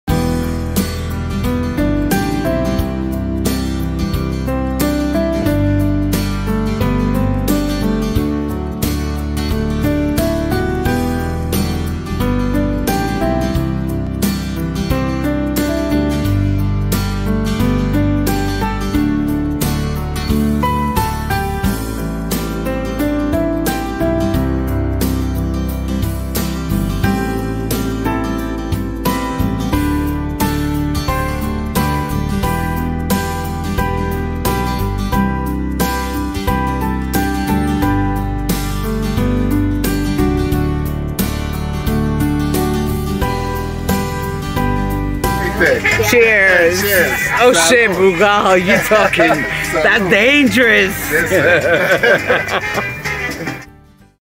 labor day with family an friends
best outing ever since busting my ankel. spending time with my children...